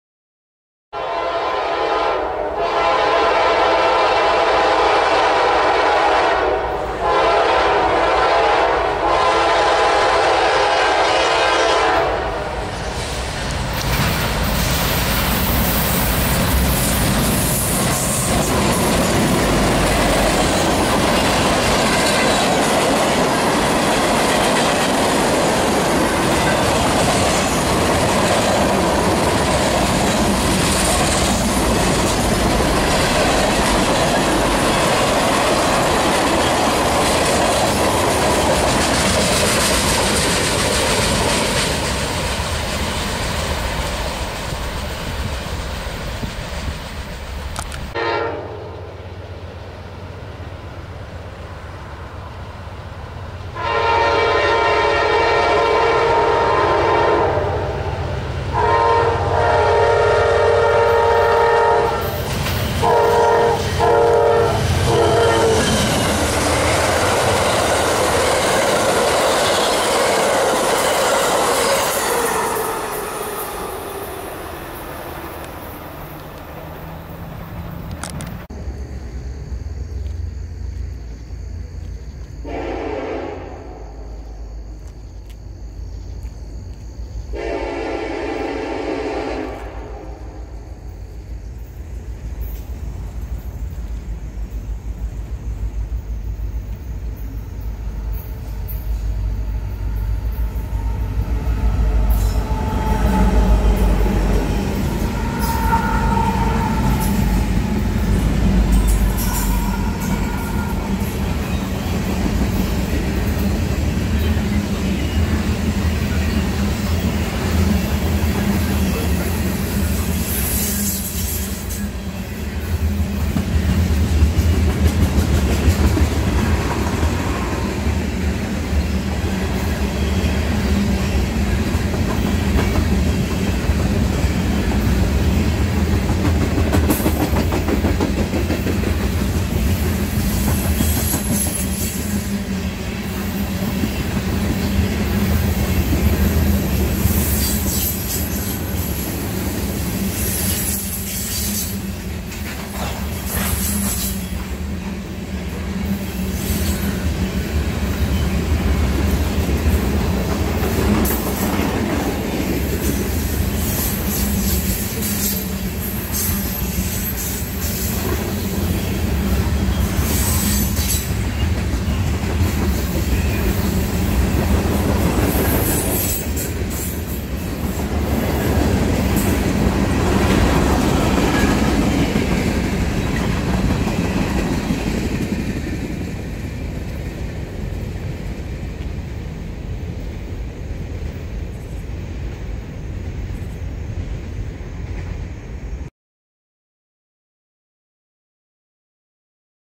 NS H-Line: Labor Day Railfanning w/SD70ACC (Garner, NC 9/2/19)
Caught 3 trains today, some missed, but couldn't be helped.
Hope you enjoy seeing 349 again on my channel!